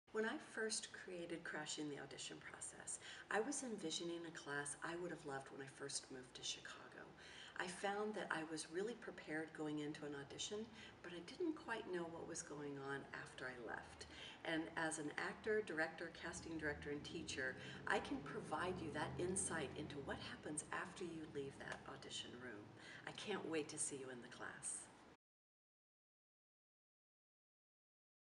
Linda Gillum teaches Crashing the Audition Process at Studio Bumppo
Crashing the Audition Process at Remy Bumppo's Studio Bumppo

Do you wonder about what happens in the audition room after your audition? What can you control and what can you let go? Remy Bumppo Core Ensemble member Linda Gillum will demystify the audition process. As an Equity Actor, Director, and Casting Director Linda has a unique insight into the casting process.

This class provides four sessions where students will work on monologues and various cold reading sides for callbacks. You will receive coaching and feedback on your work with an emphasis on the use of language. Class size is small for overall individual attention.

DATES: 4 sessions: Oct. 4, 11, 18, and 25

COST: $240